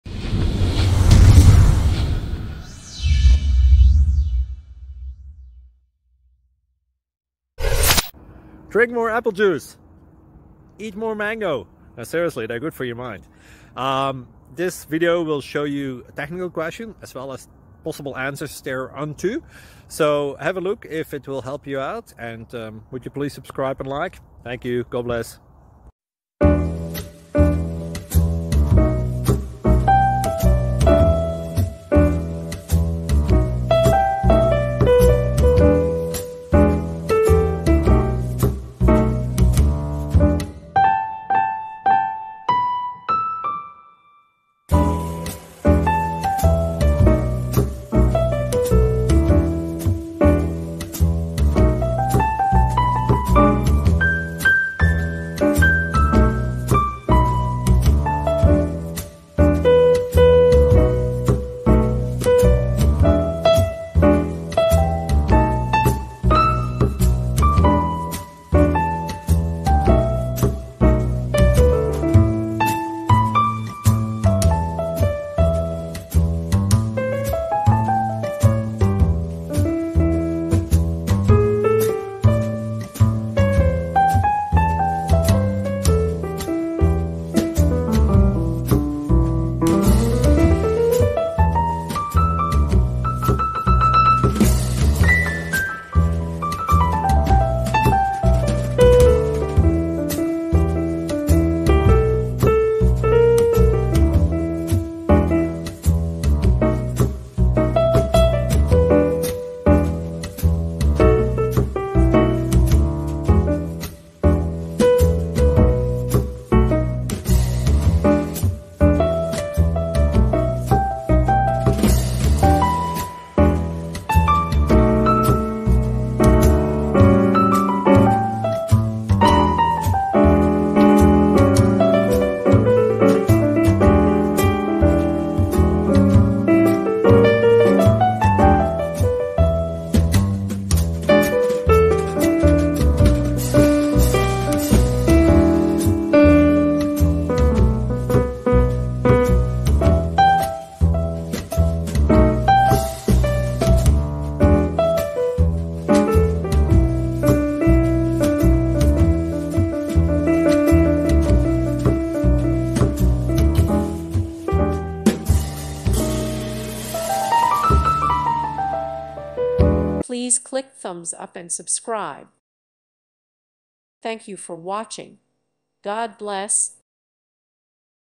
Engineering: Is Scrooge's giant glass dome feasible in real life?
With thanks & praise to God, and with thanks to the many people who have made this project possible! You are responsible for your own actions.